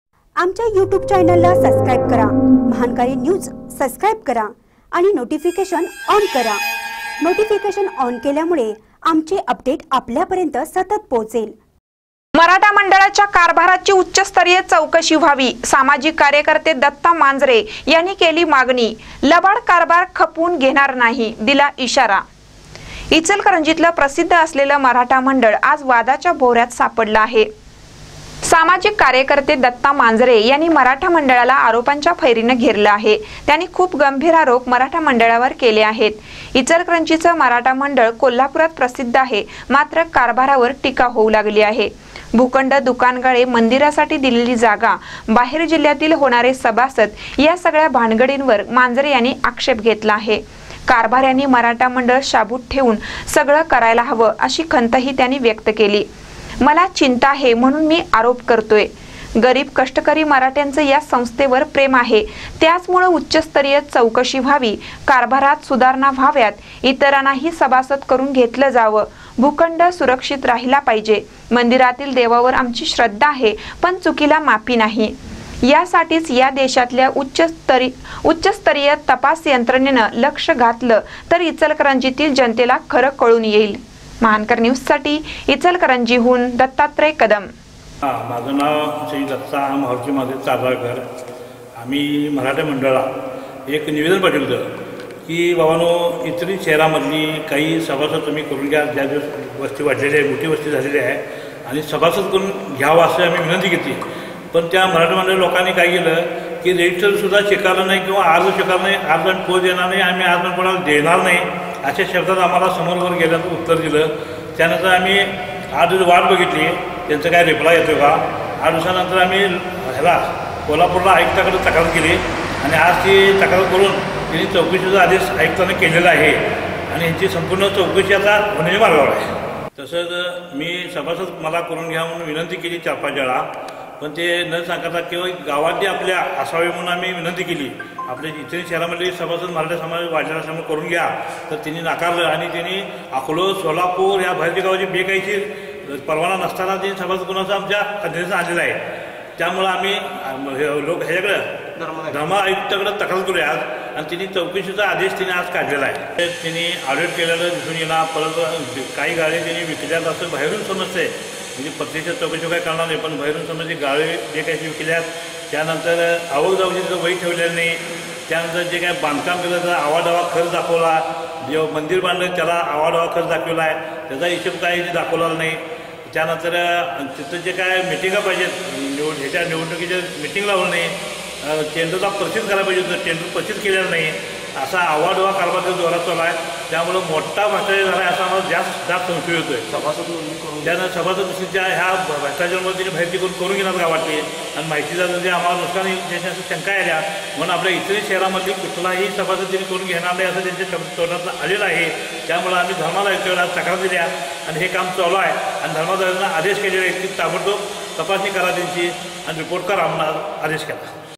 मराठा मंडळाच्या कारभाराची उच्चस्तरीय चौकशी व्हावी
मराठा मंडळाच्या कारभाराची उच्चस्तरीय चौकशी व्हावी : सामाजिक कार्यकर्ते दत्ता मांजरे यांनी केली मागणी